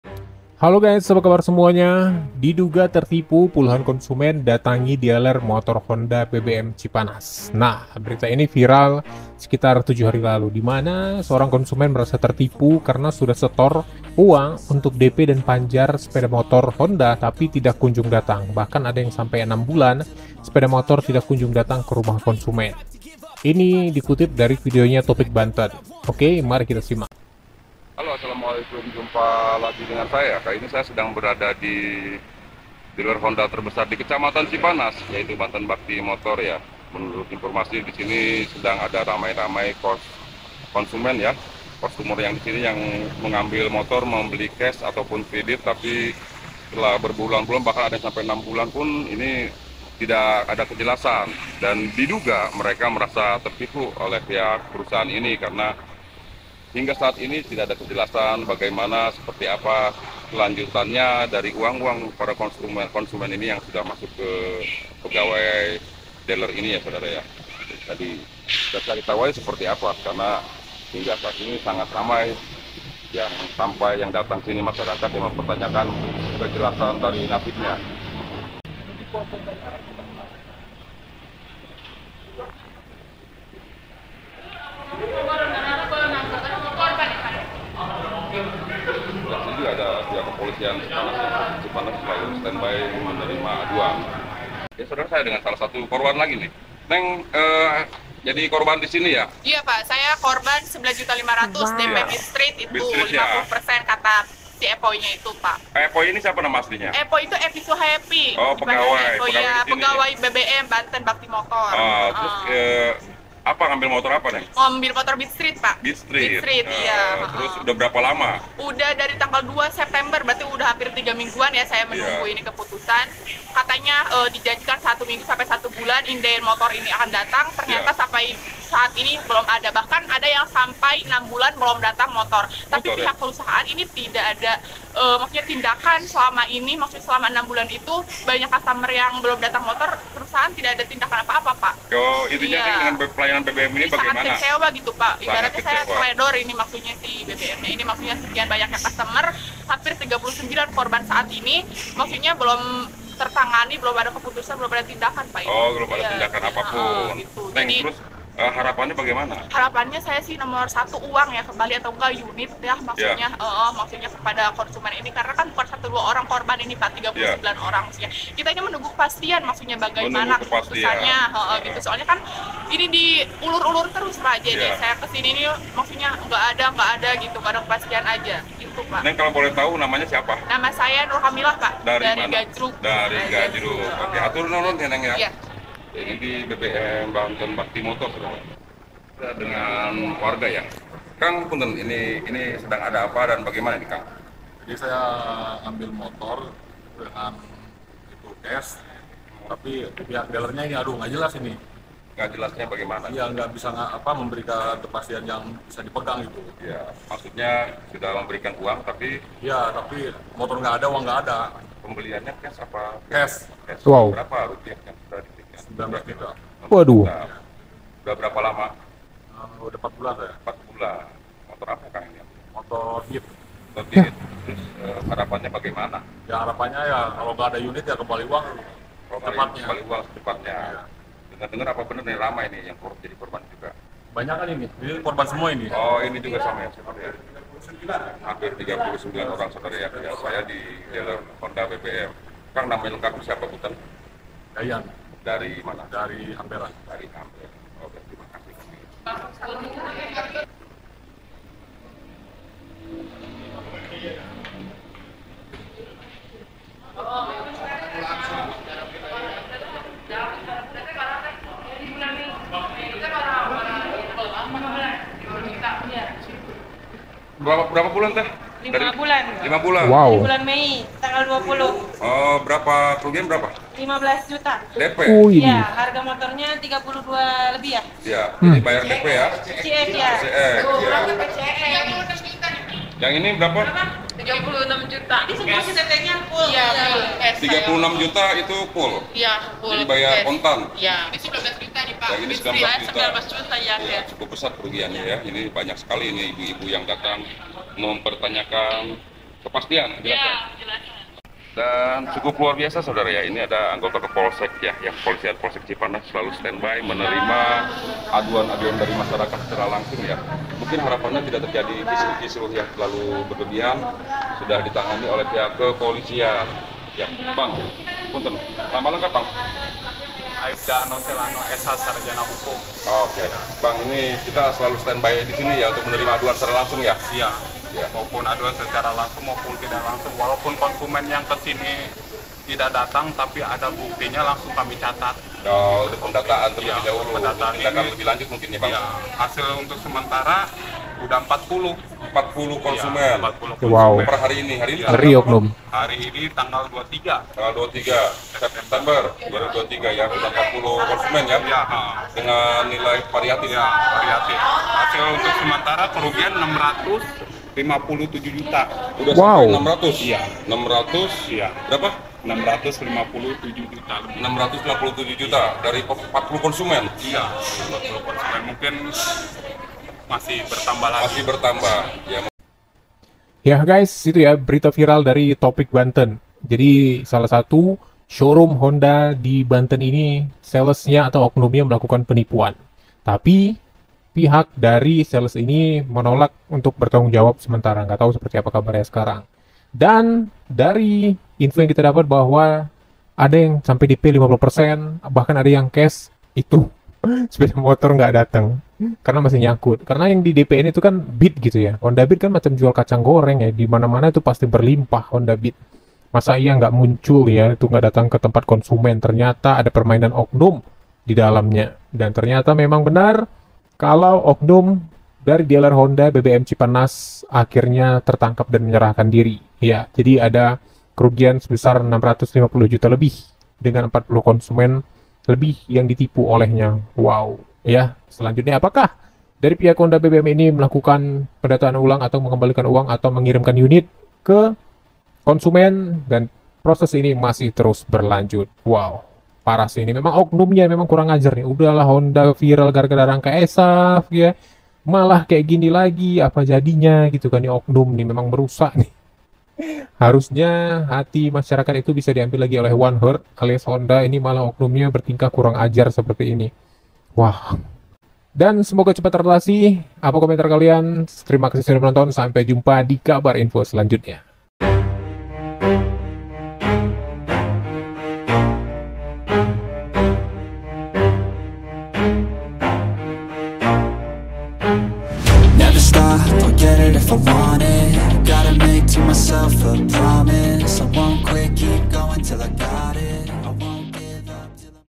PULUHAN KONSUMEN GRUDUK SHOWROOM HONDA KARENA DI TIPU TERUS MENERUS ! MOTOR DI DP TAK KUNJUNG DATANG
PULUHAN KONSUMEN GRUDUK SHOWROOM HONDA KESAL DITIPU TERUS MENERUS, SUDAH PANJAR BAHKAN BAYAR CASH UNTUK BELI MOTOT, TAPI UNIT TAK KUNJUNG DATANG KE TANGAN KONSUMEN